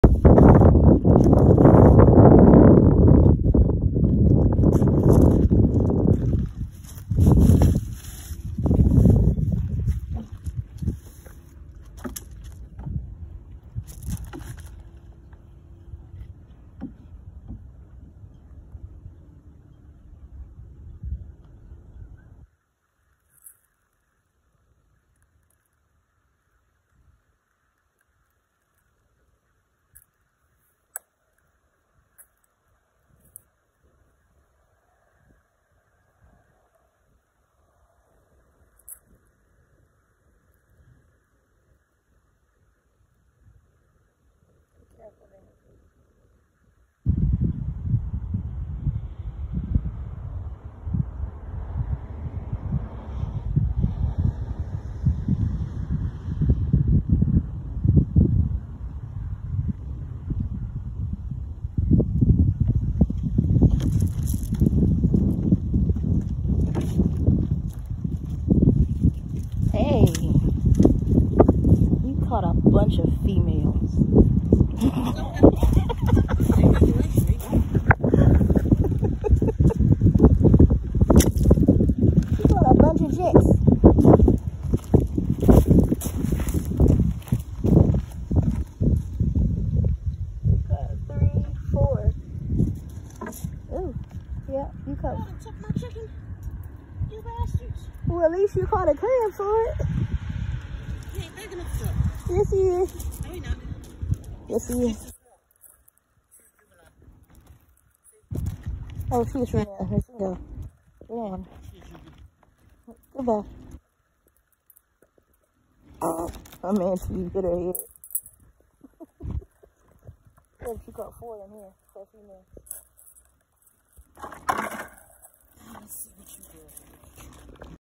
Crabbing in Island Heights NJ. West Street Crabbing Pier.
Had a great day last week at West Street Crabbing Pier in Island Heights NJ. $25.00 for a seasonal pass you can crab all you want @thandastones888 you have to try this spot out as well.

Happy Crabbing